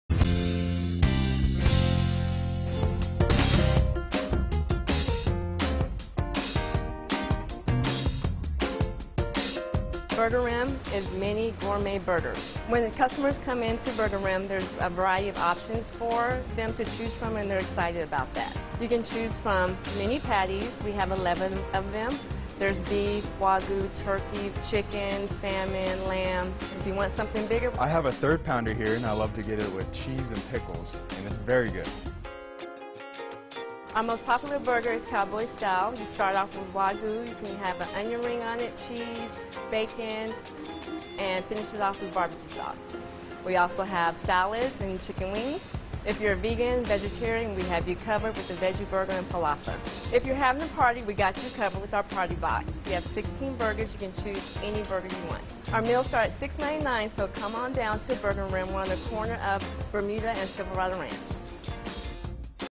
Burgerim features mini gourmet burgers
Burgerim is located near Bermuda and Silverado Ranch